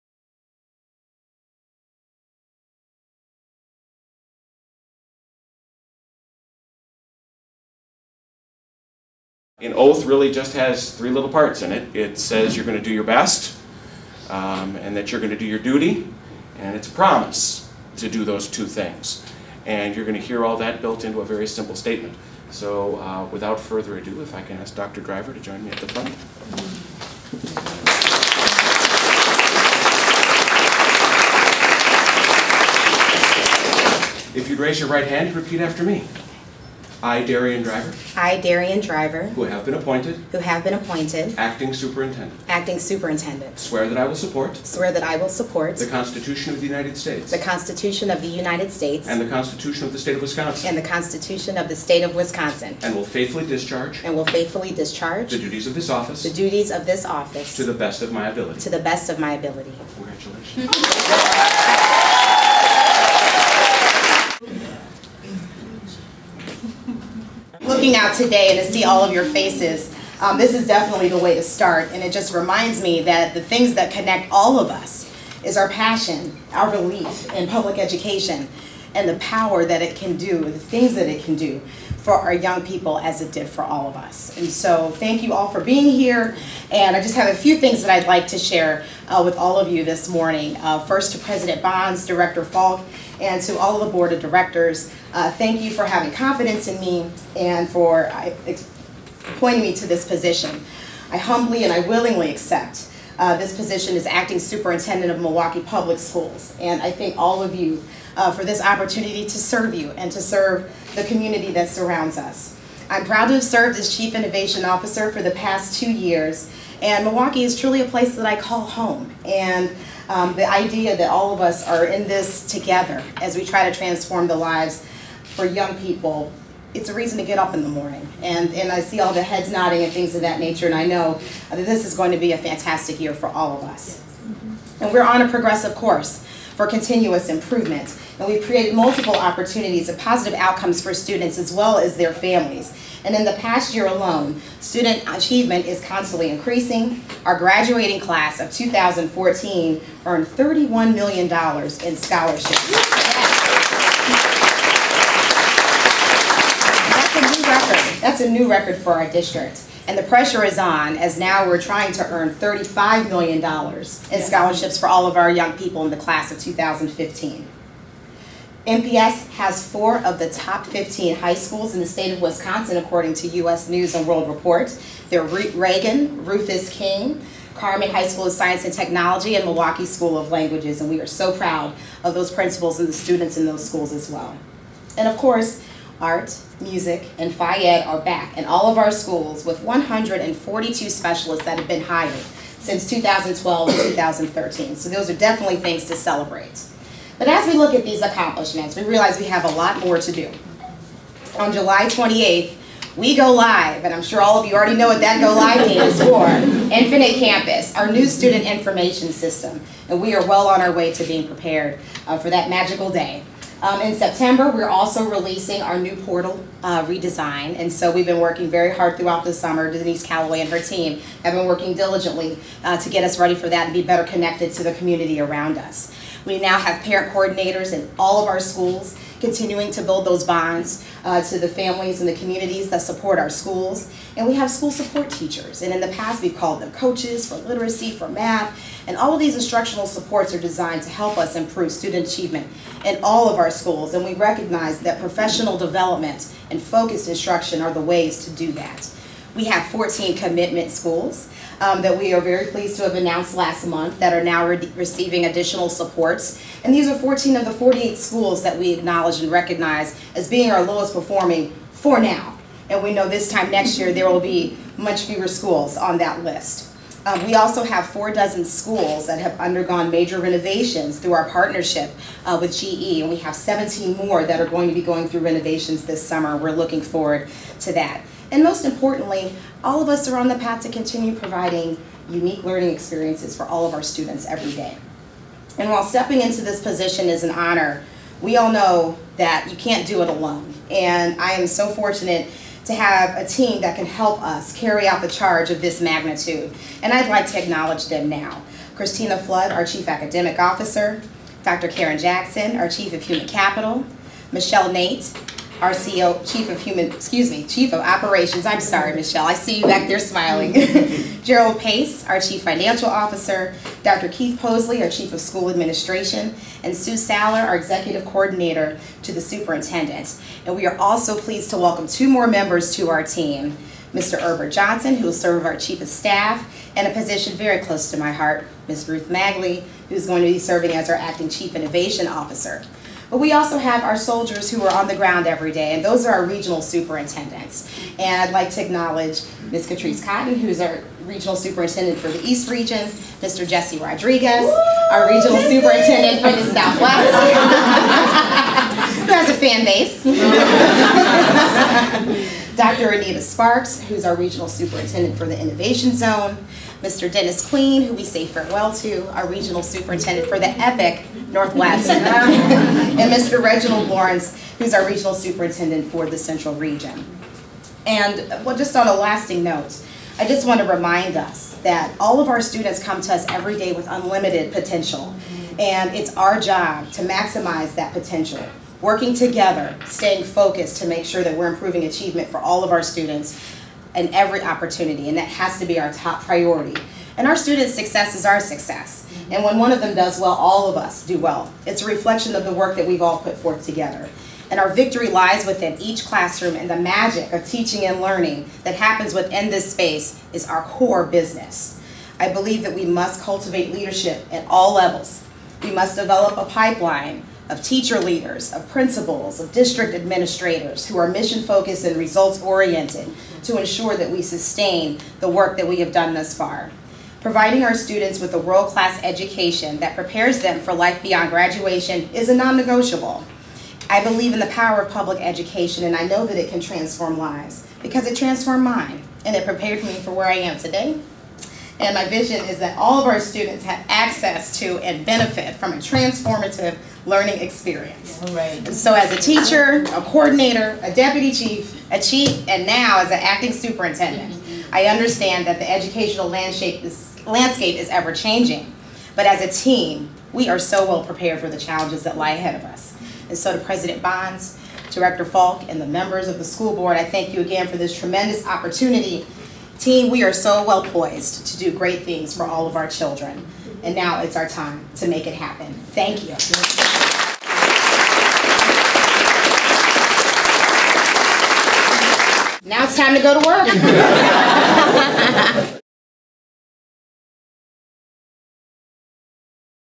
Dr. Darienne Driver sworn in as MPS Acting Superintendent
Dr. Darienne Driver was sworn in as Acting Superintendent of Milwaukee Public Schools July 1. Dr. Driver was joined by members of the Milwaukee Board of School Directors and senior district leadership.